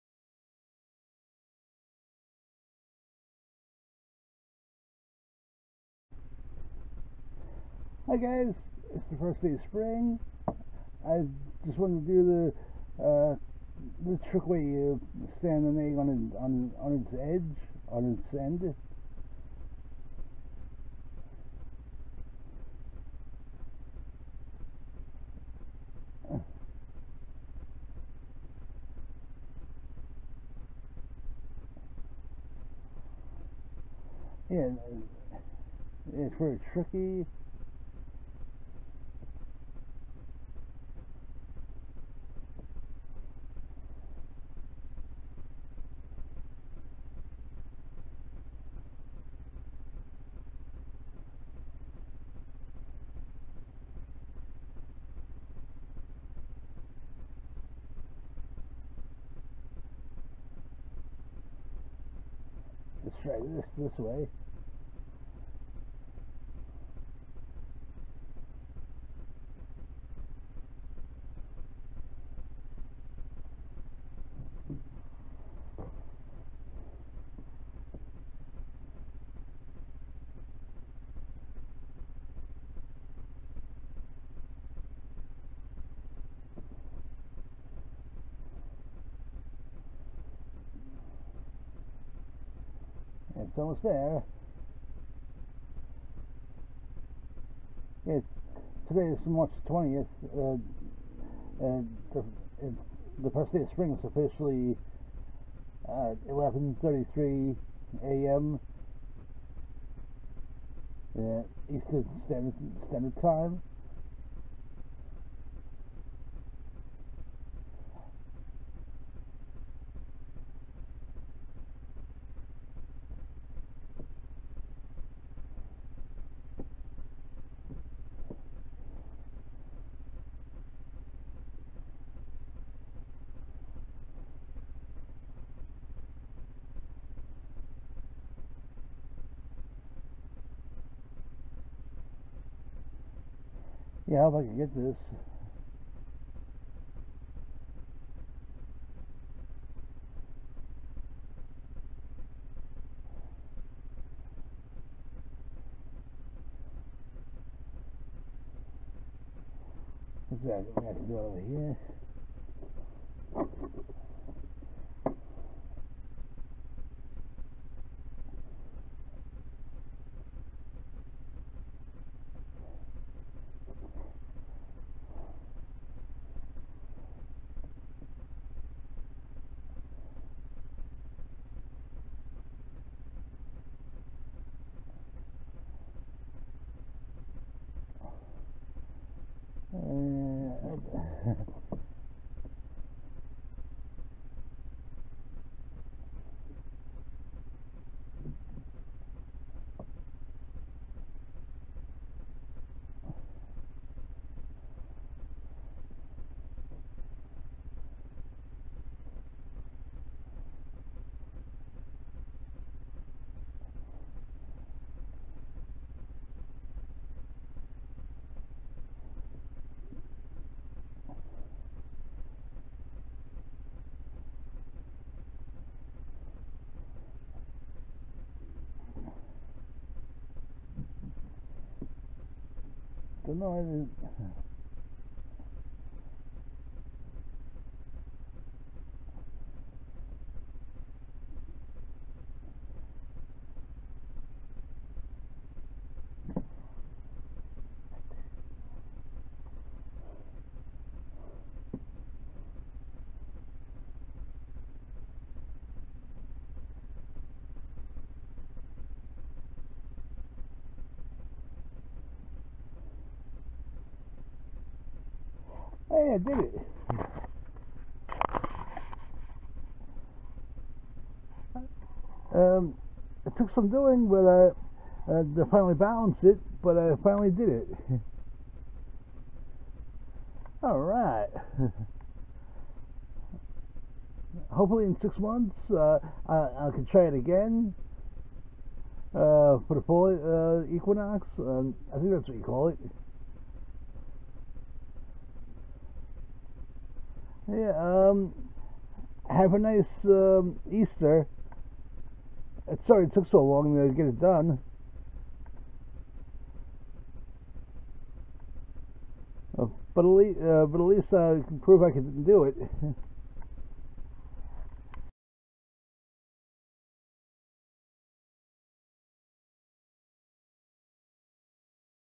First Day of Spring with an Egg
Spring Equinox 2022 in Northern Hemisphere was at 11:33 AM on
Sunday, March 20 2022. I tried to balance an egg on its end. Sorry it took so long to do it and for the sound quality.